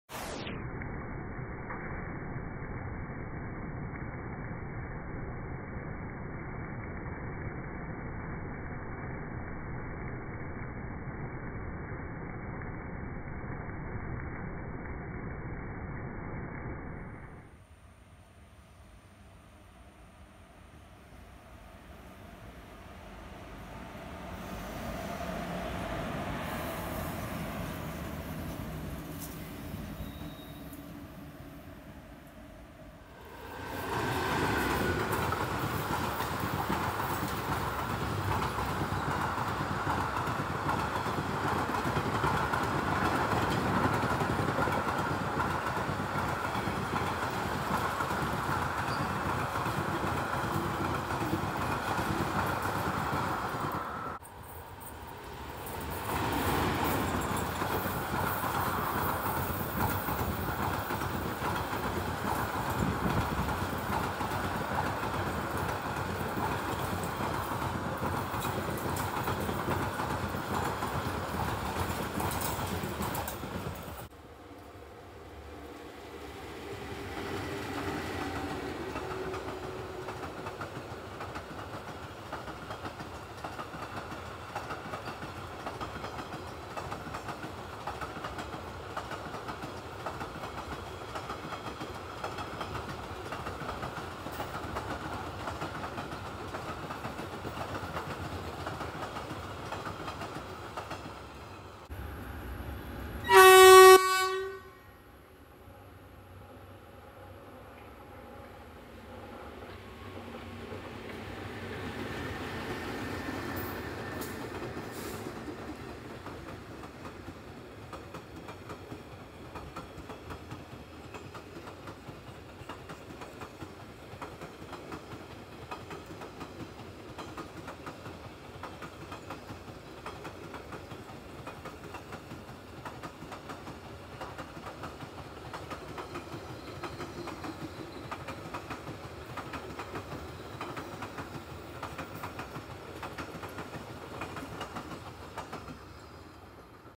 4 in 1 compilation of trains captured in Rains Monsoon revisited Kerala
INDIAN RAILWAYS

17230/Sabari Express (PT)
శబరి ఎక్స్‌ప్రెస్ ശബരി എക്സ്പ്രസ്സ്‌     शबरी एक्सप्रेस

16649/Parasuram Express
ಪರಶುರಾಮ್ ಎಕ್ಸ್‌ಪ್ರೆಸ್     परशुराम एक्सप्रेस

Kochuveli Ernakulam Hyderabad

16346/Netravati Express (PT)
നേത്രാവതി എക്സ്പ്രസ്സ്‌     नेत्रावती एक्सप्रेस